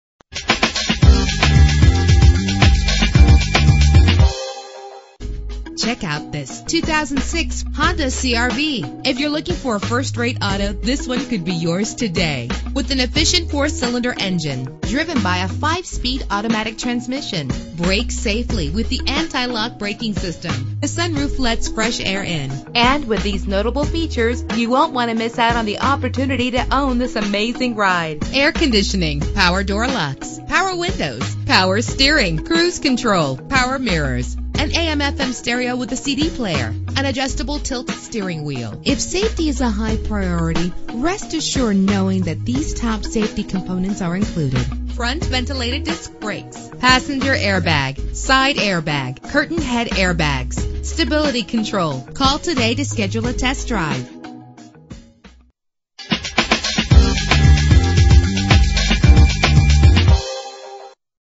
2006 Honda CR-V - Bedford OH
Year: 2006
Make: Honda
Model: CR-V
Trim: EX
Engine: 2.4 liter inline 4 cylinder 16 valve
Transmission: 5-Speed Automatic
Color: Nighthawk Black Pearl
Mileage: 37890
Address:
333 Broadway Ave